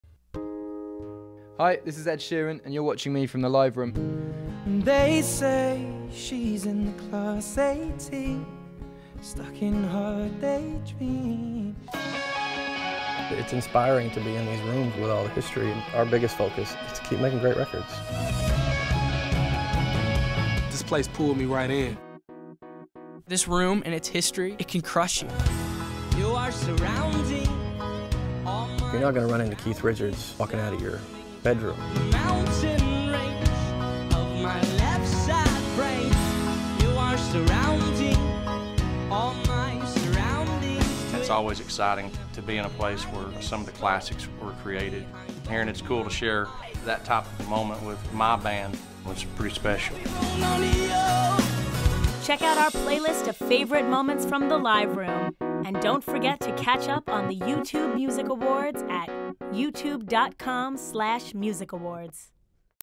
The Live Room Channel Trailer
The Live Room powered by Warner Music, is an exclusive series that captures today's most innovative artists performing live recording sessions in the most iconic recording studios across America.

Featured artists:
Ed Sheeran
T.I.
twenty one pilots
Blake Shelton
Goo Goo Dolls
B.o.B
Christina Perri
Shinedown
Halestorm
Sheryl Crow
Regina Spektor
Gary Clark Jr.
Dwight Yoakam
Jesse y Joy